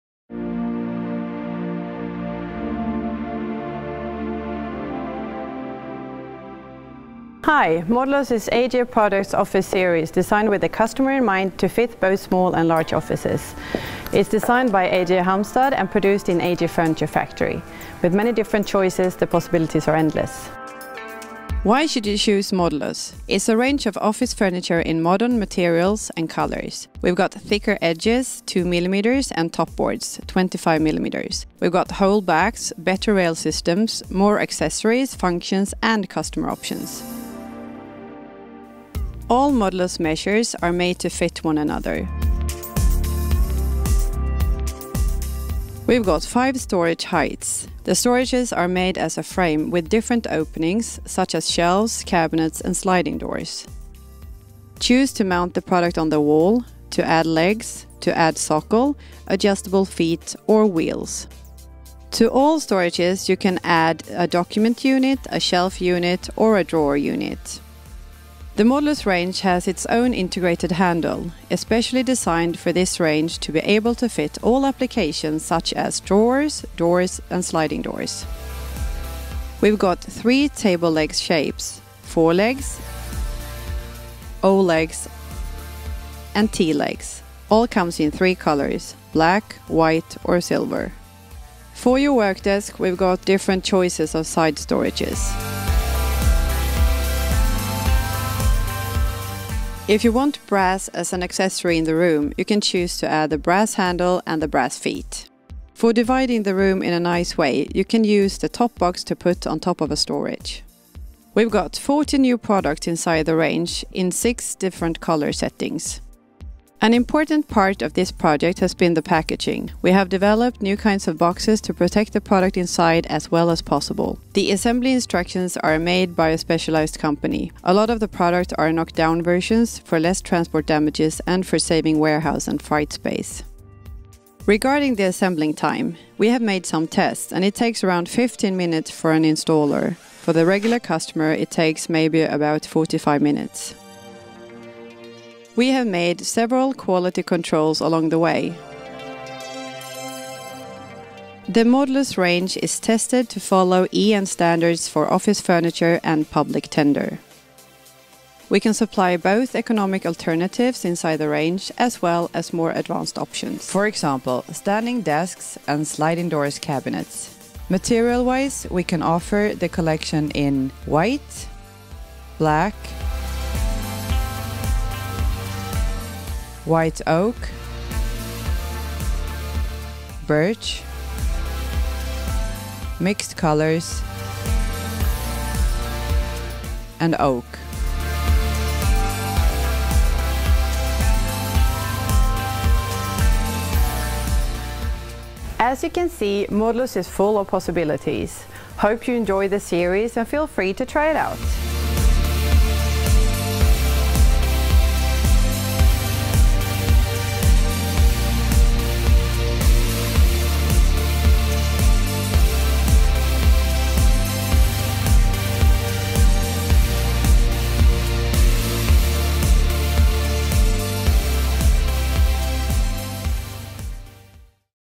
MODULUS - DESIGN DITT EGET KONTOR
"Tanken bak Modulus var å designe en serie som enkelt kan tilpasses etter behov. Oppbevaringsmodulene består av et rammeverk som du kan fylle med innredningsløsninger og skuffer som passer deg".

Elin Badansdre André, designer av Modulus.